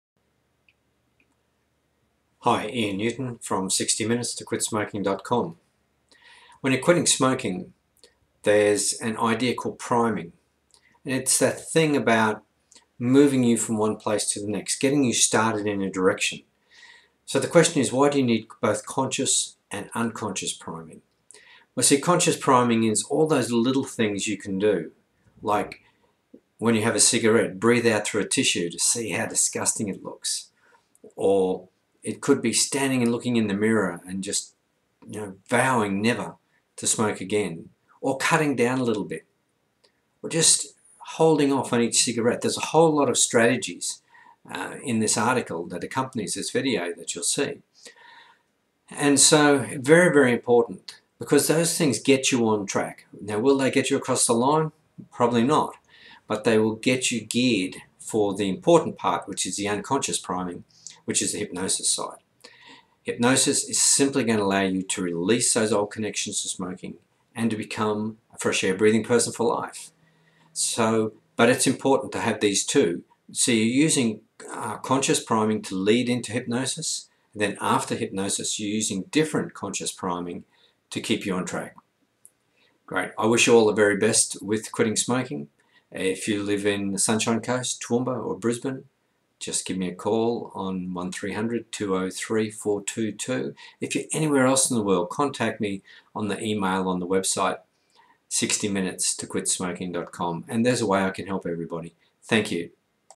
Why You Need Both Conscious & Unconscious Priming To Help you Quit Smoking
Ian Newton iChange Priming expert explains why you need both you conscious and unconscious mind to be primed for successfully quitting smoking.